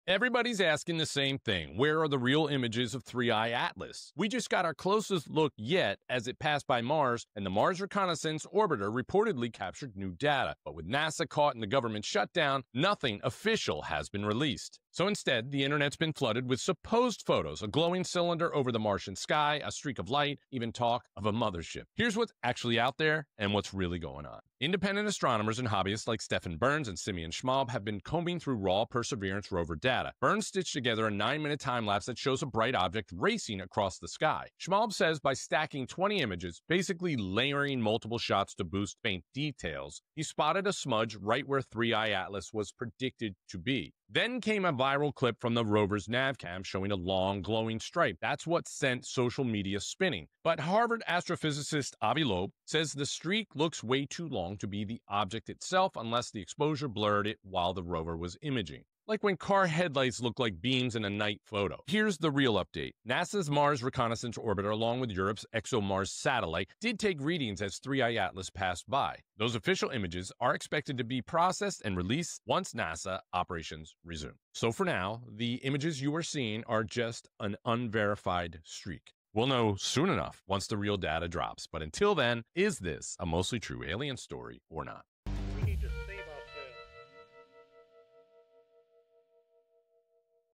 Where Are the Real 3I/ATLAS Images?
Everyone’s asking the same question: where are the real images of 3I/ATLAS?

The interstellar object just passed Mars, and while the Mars Reconnaissance Orbiter captured new data, NASA’s shutdown means nothing official has been released yet.
Instead, the internet’s full of “leaked” photos — glowing streaks, supposed motherships, and wild theories.

Here’s what’s real:

Independent astronomers stitched and stacked rover data to spot a moving object

Viral clips show glowing streaks in the Martian sky

Experts like Harvard’s Avi Loeb say it’s likely just motion blur — not a mothership

The real NASA and ExoMars images should drop once operations resume — and that’s when we’ll know if 3I/ATLAS is just rock and ice… or something we’ve never seen before.

👉 What do you think — blurred comet, or proof of something bigger?